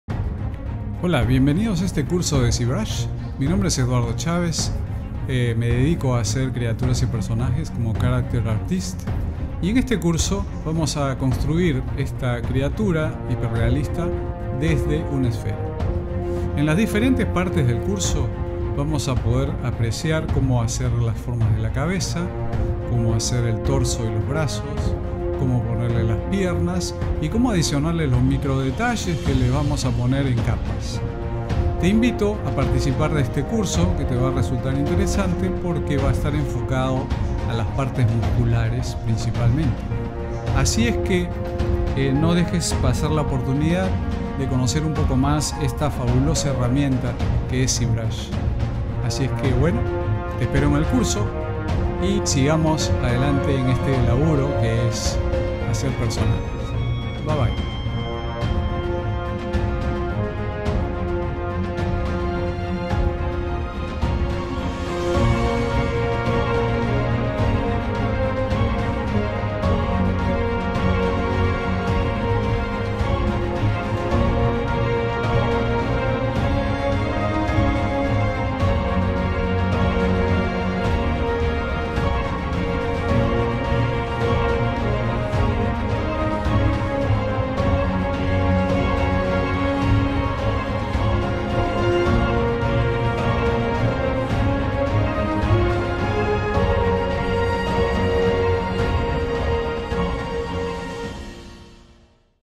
ZBrush 2020 - Esculpido de criaturas hiperrealistas
Aprende a esculpir creaturas realistas de la mano de un experto utilizando ZBrush.

Te llevaremos mostrándote todo el proceso, desde crear las formas bases con Zspheres, hasta ajustar escala y pose del personaje, posteriormente agregaremos la estructura muscular basada en anatomía real para dotar de credibilidad al personaje, agregaremos garras, dientes, así como detalles mas finos en la piel, escamas y cicatrices, finalmente haremos un render directo en ZBrush para presentar el personaje final.

Un curso para quienes ya tienen un conocimiento básico a intermedio en ZBrush.